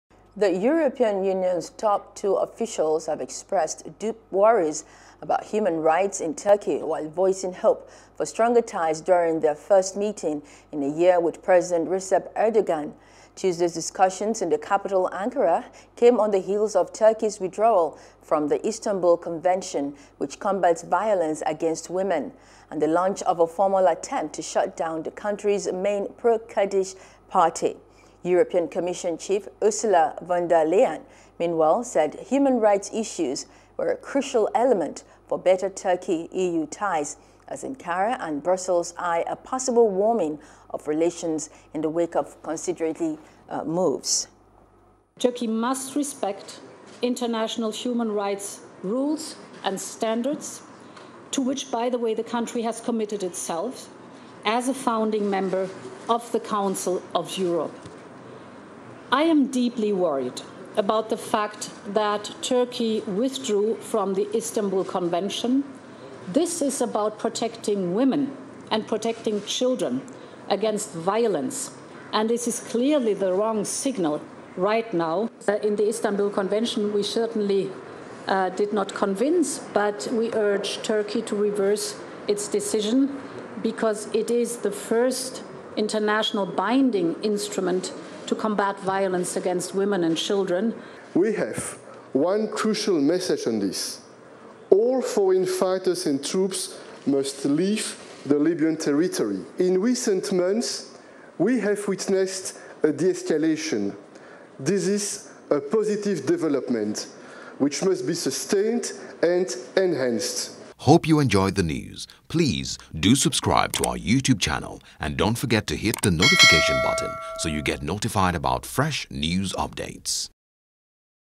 EU Commission To Make New Proposal To Turkey On Refugee Support | FOREIGN
The European Union’s top two officials have expressed deep worries about human rights in Turkey while voicing hope for stronger ties during their first meeting in a year with President Recep Erdogan.